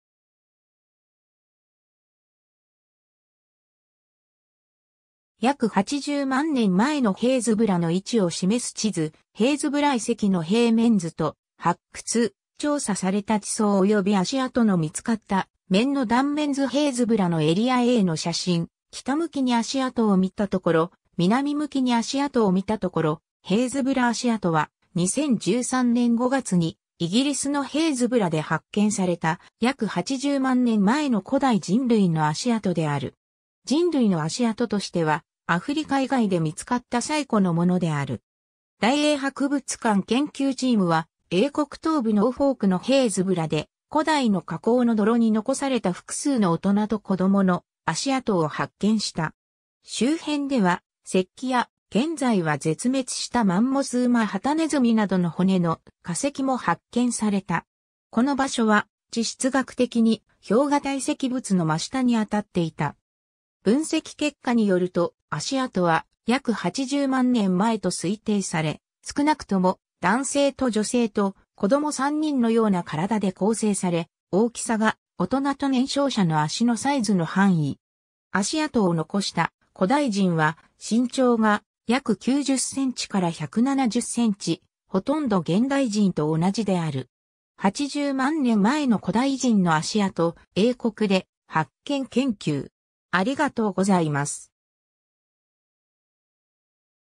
ヘイズブラ足跡
約80万年前（更新世）のヘイズブラの位置を示す地図 ヘイズブラ遺跡の平面図と、発掘・調査された地層および足跡の見つかった面の断面図 ヘイズブラのエリアAの写真：(a) 北向きに足跡を見たところ、(b) 南向きに足跡を見たところ ヘイズブラ足跡（ヘイズブラそくせき、英語：Happisburgh footprints）は、2013年5月にイギリスのヘイズブラで発見された、約80万年前の古代人類の足跡である。
人類の足跡としては、アフリカ以外で見つかった最古のものである。
 大英博物館研究チームは、英国東部ノーフォークのヘイズブラで、古代の河口の泥に残された複数の大人と子どもの足跡を発見した。
周辺では石器や、現在は絶滅したマンモス・ウマ・ハタネズミなどの骨の化石も発見された。
この場所は地質学的に氷河堆積物の真下に当たっていた。
 分析結果によると、足跡は約80万年前と推定され、少なくとも男性と女性と子ども三人のような体で構成され、大きさが「大人と年少者の足のサイズの範囲」。
足跡を残した古代人は身長が約90センチから170センチ、ほとんど現代人と同じである。
 ^ 80万年前の古代人の足跡、英国で発見 研究